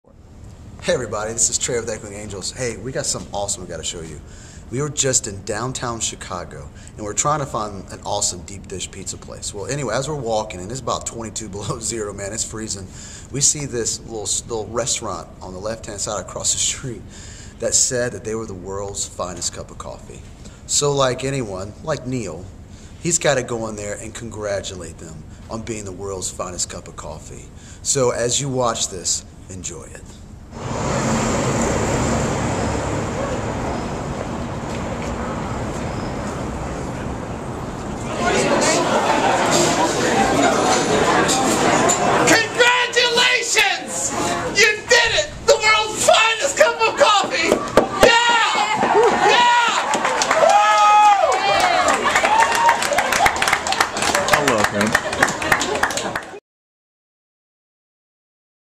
Neil and the Worlds best cup of Coffee!!
We saw a restaurant in Chicago that said it had the Worlds best cup of coffee. So we had a nice "Elf" moment.